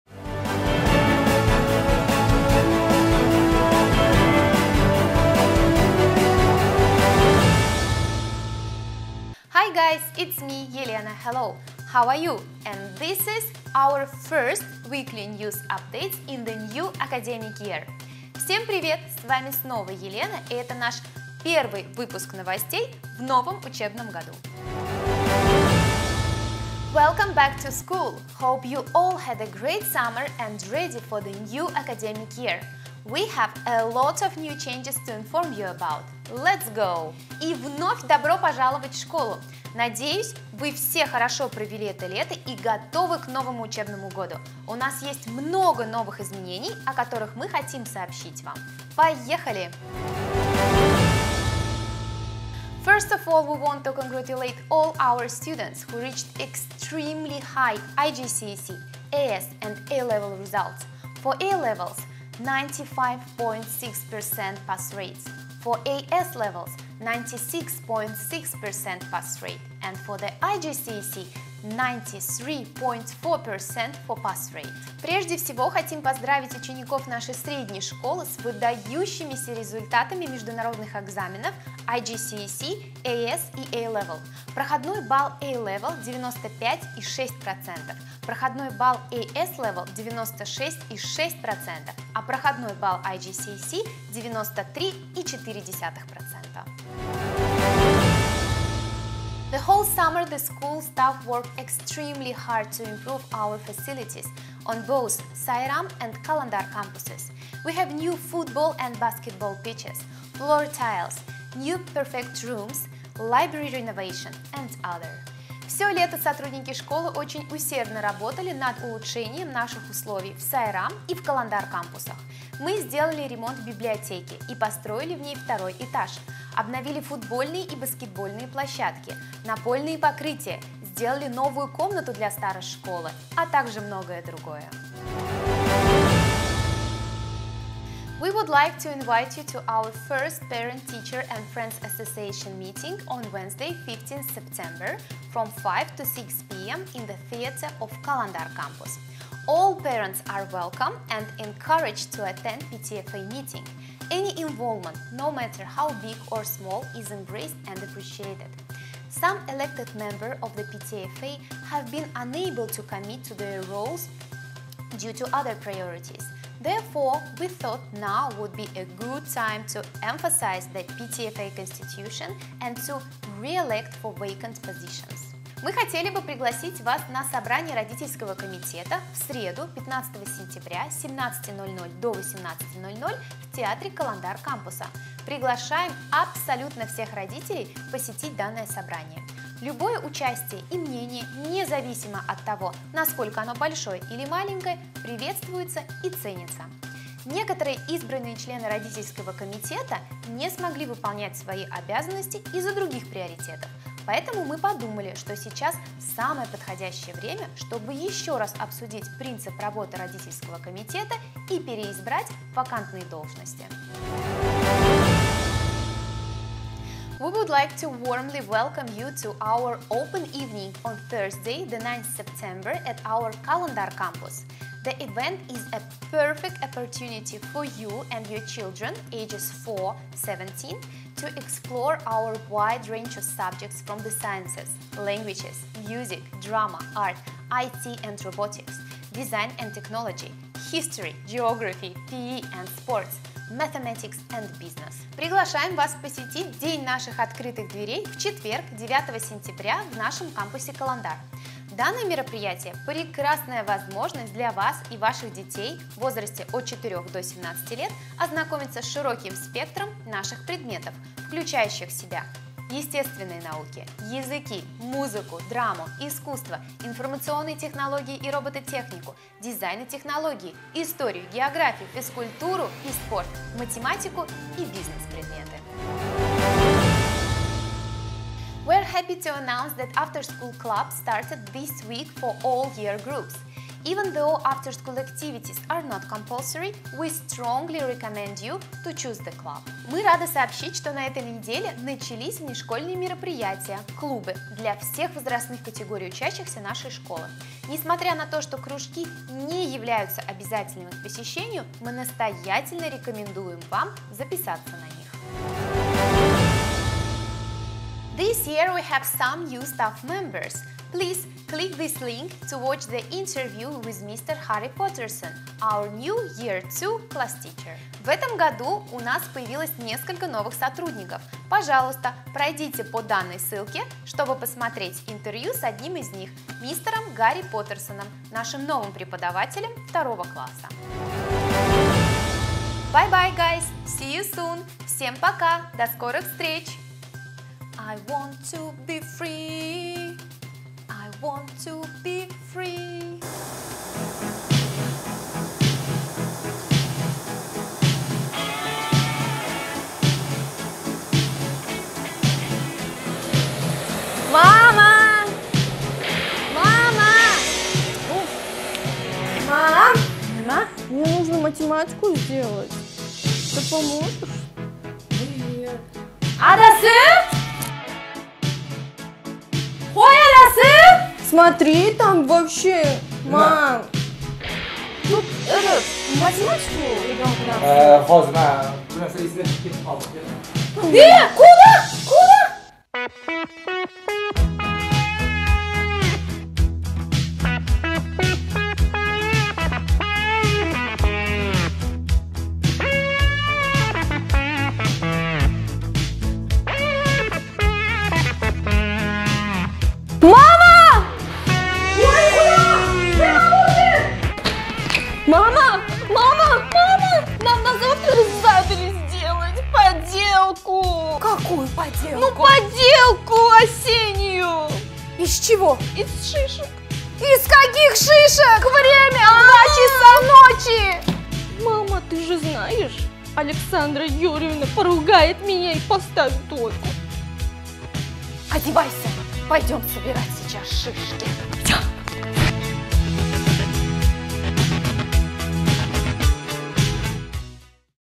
BST News Flash - 1 Edition - 2021/22 Academic Year
In this issue / В этом выпуске:

Outstanding results of our IGCSE, AS and A level
Выдающиеся результаты IGCSE, AS и A level

Improved facilities  Улучшение условий
PTFA meeting  Родительский комитет

After school clubs and extracurricular activities
Кружки и внешкольные занятия

Interview with Harry Patterson
Интервью с Гарри Паттерсоном